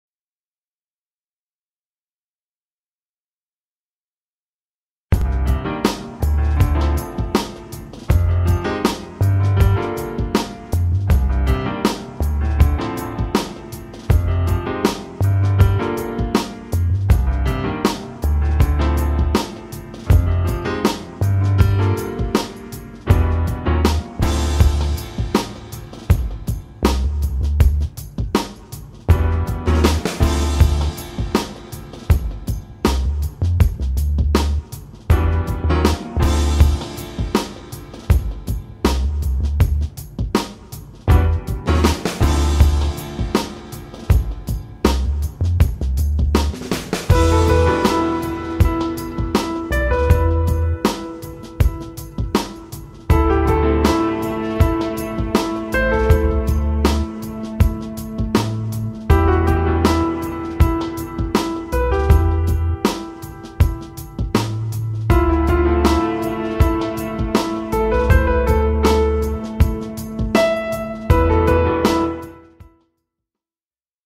Porsche 911 occasion visible à Saint-gregoire présentée par Alpine rennes
Cette Porsche 911 occasion disponible à Saint-gregoire dans le 35 est mise en vente par Alpine rennes - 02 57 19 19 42 - tous les détails sont